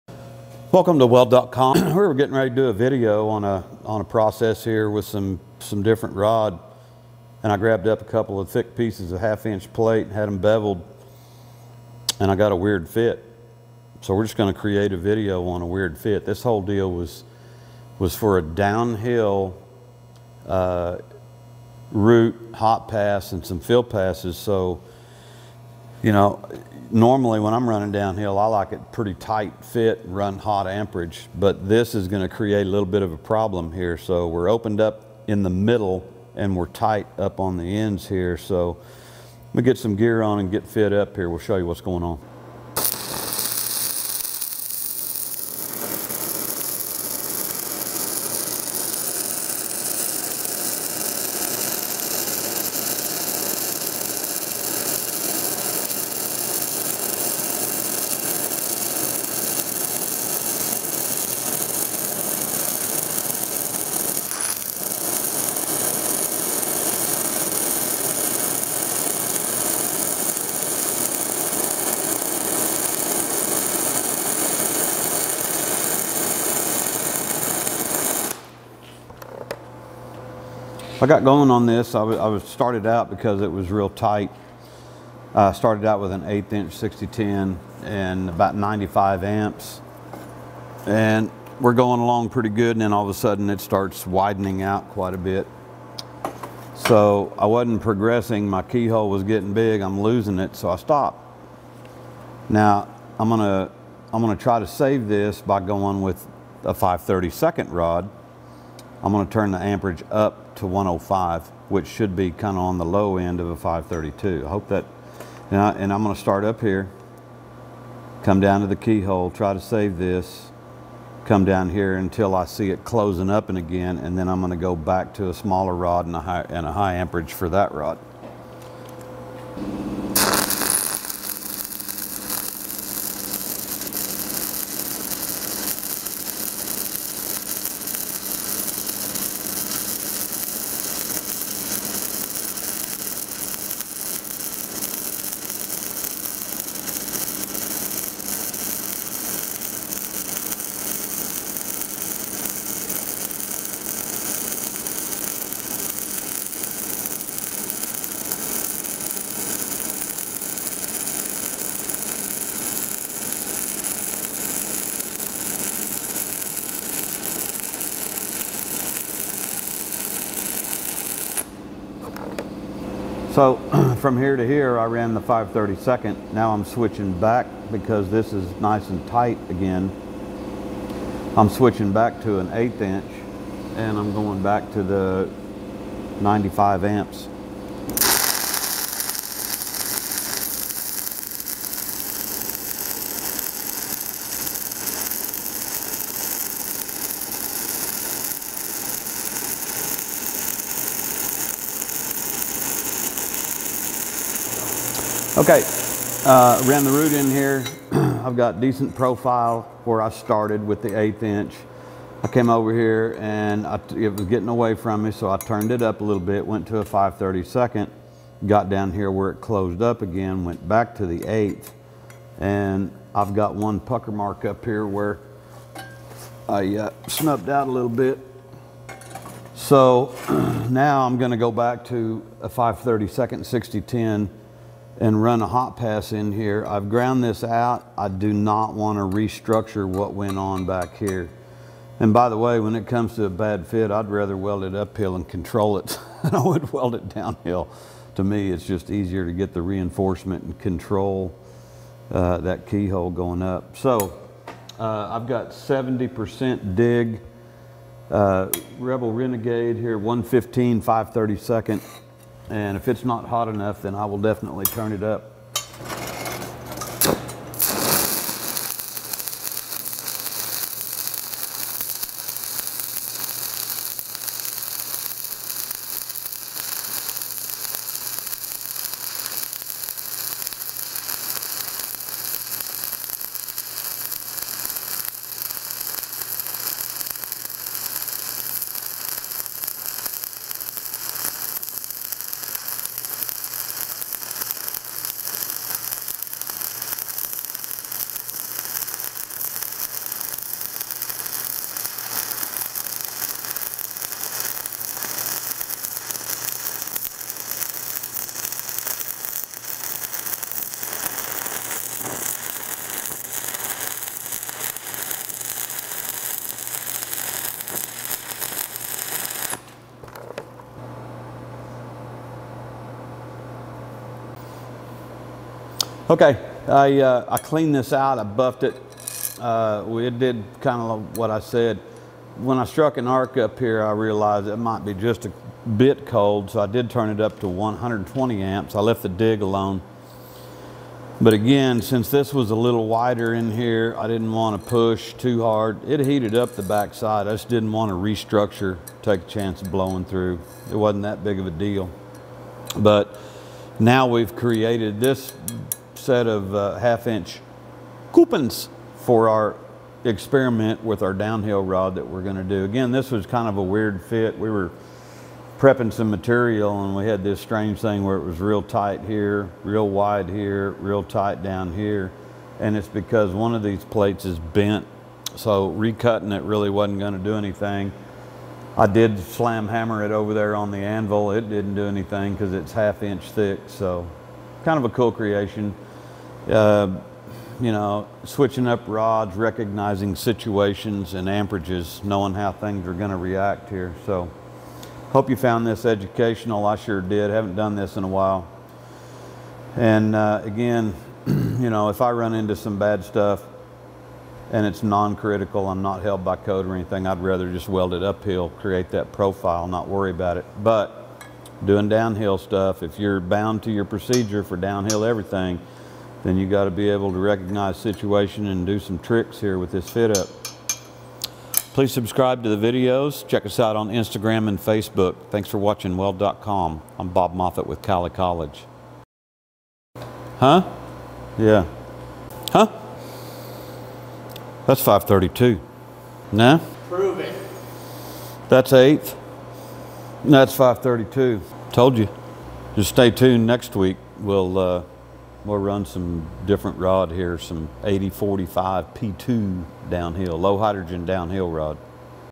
🔥 Downhill Open Root 6010 with UNEVEN FITUP
We were actually going to do a video on 8024-P2 today, but when we prepped the plate to do it we had a terrible fit. The plate was bent, so we had a nice tight gap on the ends, but the middle opened up quite a bit. Instead of scrapping it, we decided it was a great opportunity to show everyone what happens in real life and how to deal with it.

In this video we will be running downhill 6010 electrode to burn in the root. Once the opening starts to get too wide for our original stick, we will step it up in electrode diameter and amperage to fill that additional gap. Once we get back to a tight fit, we'll go back to a smaller electrode and lower our amps.